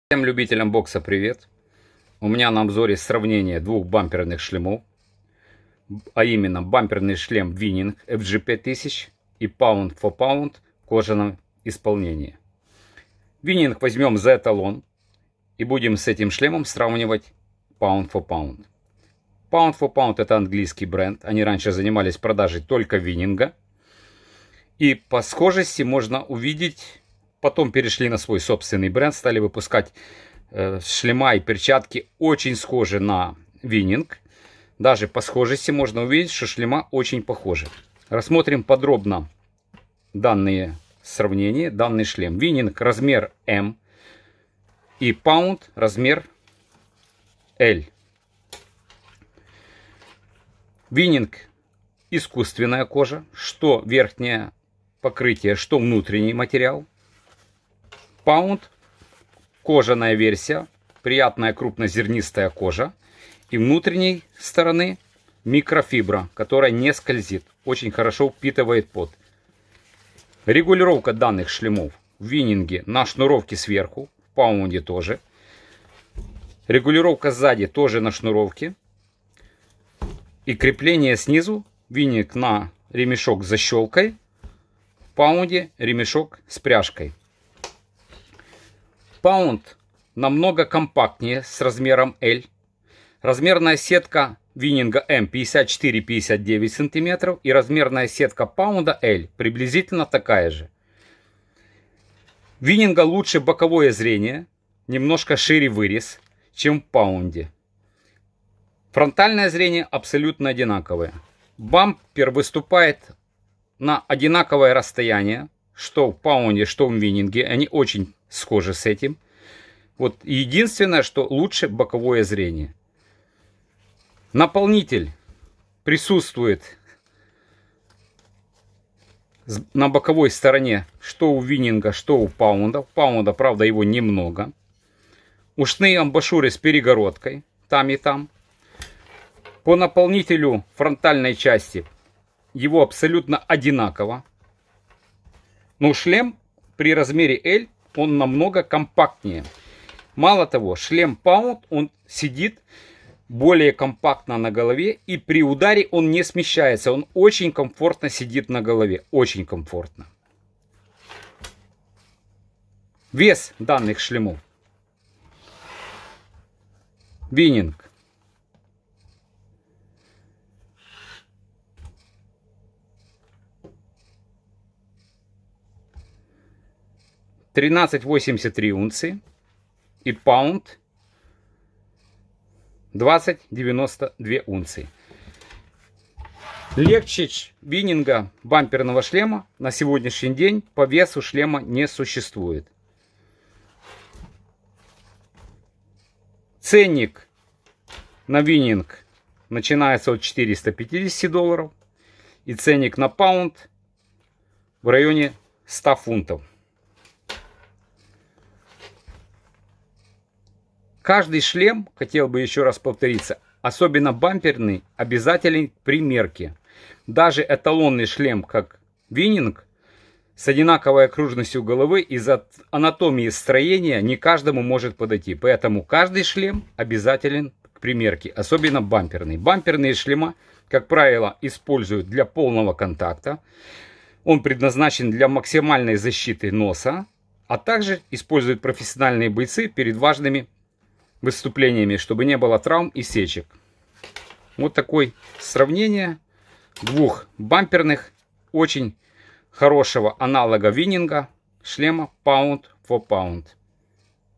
Full Face Pound 4 Pound L и WINNING FG-5000 Full Face Training Headgear -Обзор и сравнение
Боксерский шлем Full Face Pound 4 Pound Headgear
Лучший аналог боксерского шлема Winning FG 5000 только в кожаном исполнении.Цена 125 фунтов.Шлем очень компактный нет выступающих элементов.По визуальному ощущению похож на подростковый, хотя по внутреннему удобству не маленький, идеальная анатомическая посадка
WINNING FG-5000 Full Face Training Headgear made in Japan -Обзор
Японский бамперый шлем WINNING FG-5000 для тренировочного спарринга.
Описание:
Ручная работа из Японии!!!
Бамперый шлем для бокса WINNING гарантируют наивысшее качество защиты лица и головы спортсмена
Классическая модель защитного шлема для тренировочного спарринга
Шлем оснащено более широким отверстием, что обеспечивает значительно лучший обзор и облегчает дыхание
Передний бампер выполнен из твердого пластикового материала и способно выдержать сильные удары и защитит лицевую и носовую кость
Пластиковая прослойка покрыта сверху толстым слоем наполнителем из пены
Ушные раковины от ударов защищены надежными протекторами
Благодаря абсорбционному эффекту защитная многослойная пена эффективно снижает и рассеивает ударную волну
Внешнее и внутреннее покрытие шлема выполнена из высококачественной PU кожи высшего качества
Плотная PU кожа обладает отличной устойчивостью против повреждений и изнашивания
По своим потребительским качествам PU кожа практически не уступает натуральной и гарантирует долгий срок эксплуатации
Наполнитель: ударопрочная многослойная пена толщиной 3.5 см
Бампер: пластиковая рама
Материал: высококачественная PU кожа
Цвет: синий
Вес шлема: 13.8 унций
Размеры (обхват головы): Medium 54-59 см